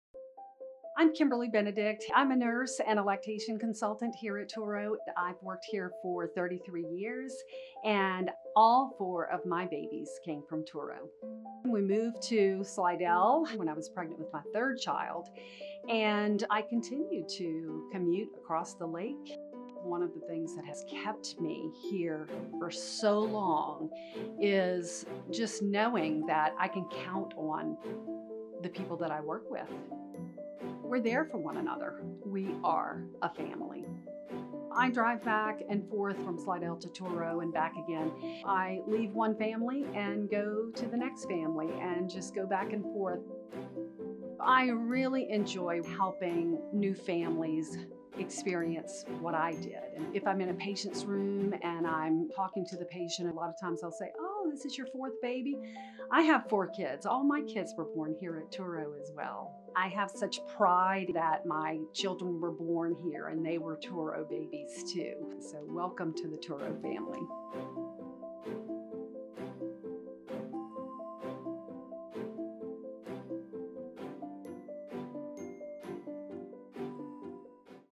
Kimberly | 100 years of Touro stories
Since 1922, Touro has been the hospital "where babies come from" in New Orleans.

There are countless stories to be told about the babies born at Touro over the past century. Meet Kimberly. She is a Touro mom of four as well as a lactation consultant at Touro for over 40 years. Here's her story.

Join us in celebrating 100 years of Touro babies